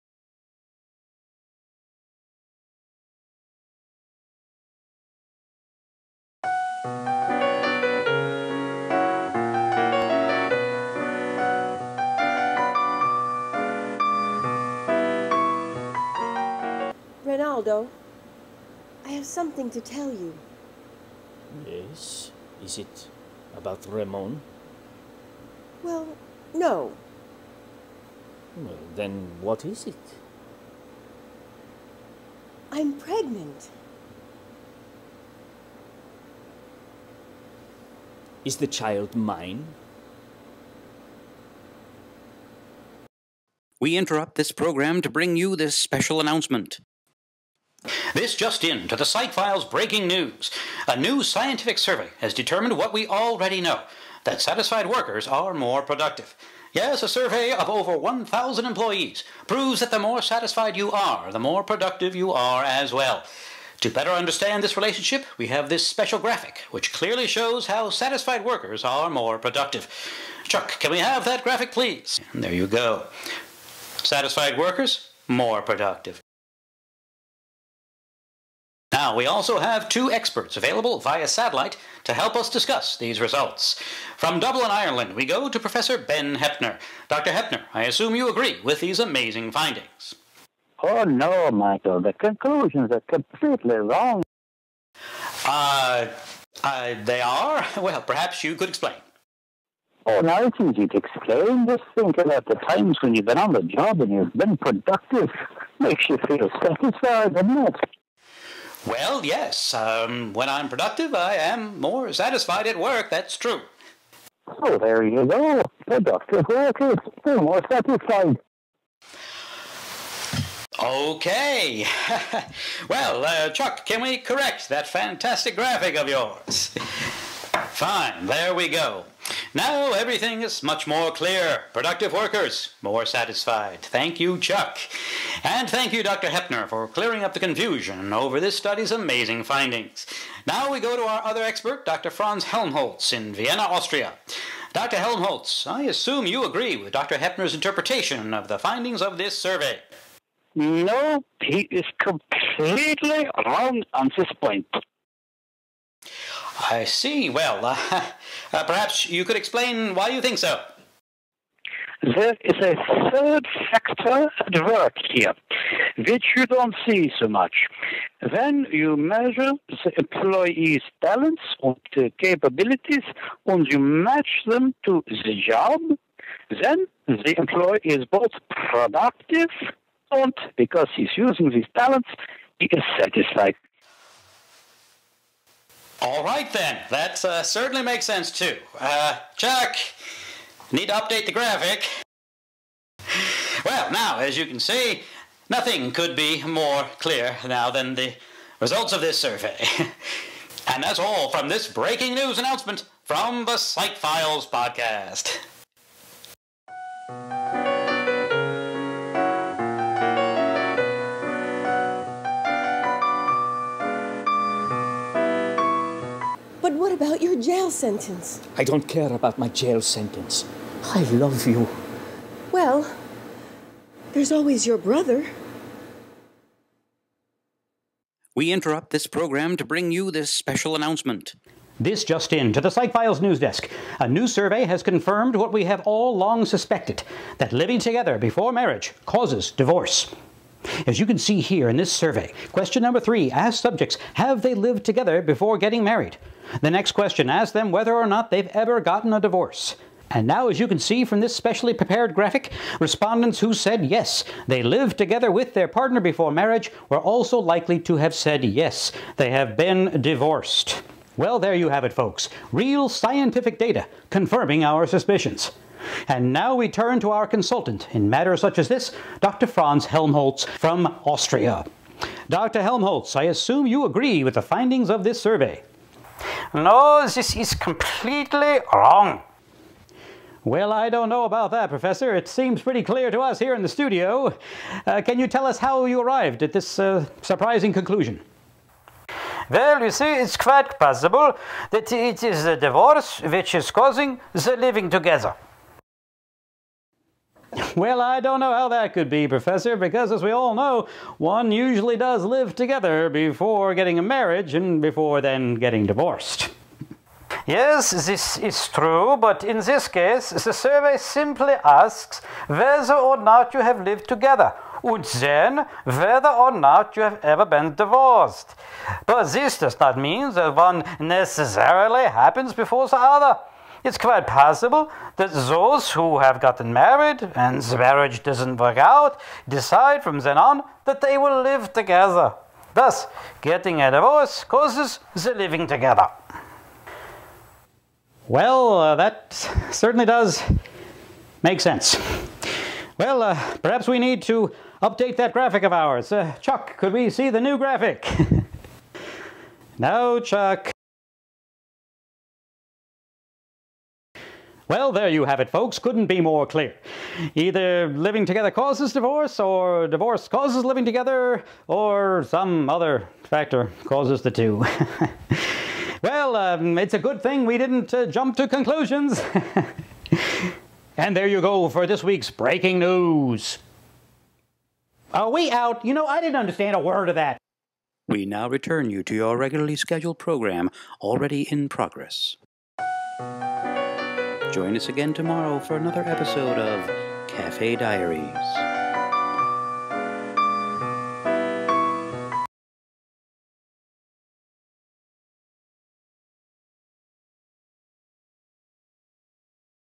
The Difference Between Correlation and Causation - a Fun Example
Looking for examples of correlation and causation? Youve heard it a million times: correlation doesnt mean causation. Still need help? Well, heres a humorous look at this topic that I think drives home the point. The Psych Files "Breaking News" explores whether satisfied workers are more productive and whether living together causes divorce. I hope you enjoy this unique video episode of The Psych Files.  Distributed by Tubemogul.